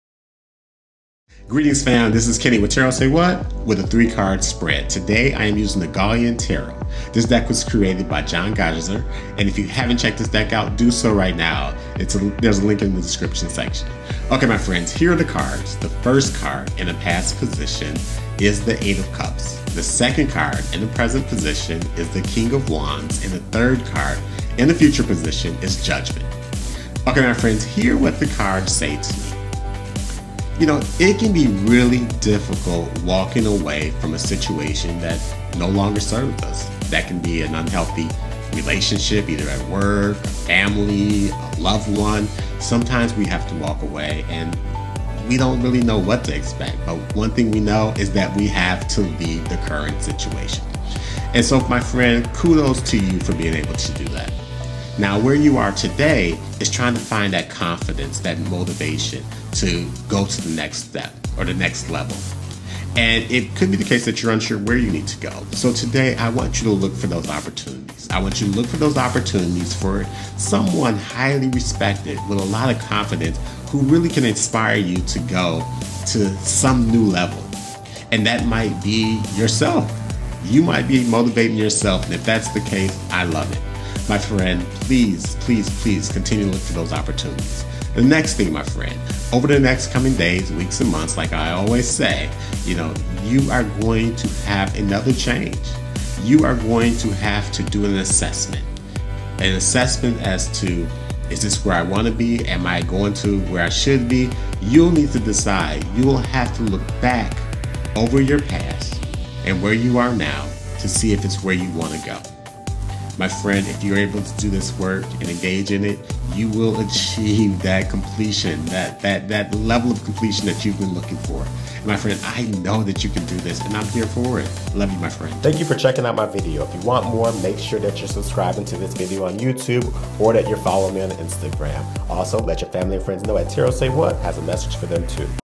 Gauean Tarot - When was the last time you respected your boundaries and left an unhealthy situation?
Everything is up for revision as you take steps to improve.

We can never be sure what we are walking into when we decide to leave unhealthy situations or relationships. It’s likely to be a mix of emotions, perhaps a roller coaster of emotions, as we learn a new normal. Today, look out for someone highly respected who motivates you to a higher and greater level of your current self. Alternatively, look within for self-motivation to go after something that lights your internal fire confidently. Soon, you’ll need to assess your current plans and path. You may find a change of plans is necessary or the need to share with others how you started on this journey.

The cards in this tarot reading came from the Gauean Tarot created by Jon Garaizar.

Tarot Say What offers daily tarot readings, usually under 90 seconds or less.